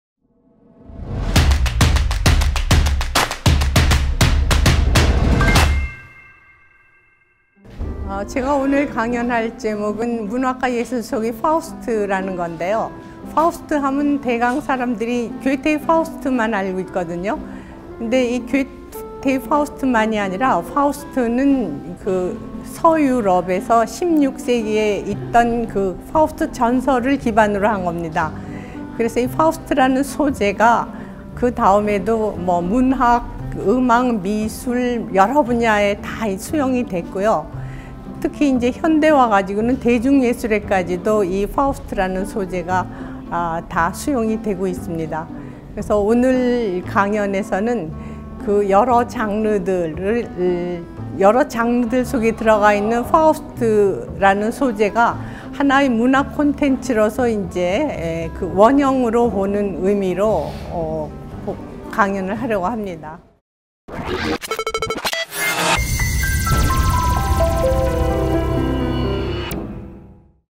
2017-2 글로벌 인문학-문학과 예술 속의 파우스트(Faust)
2017-2 글로벌 인문학 특강

문학과 예술 속의 파우스트(Faust)
윤시향(연극평론가)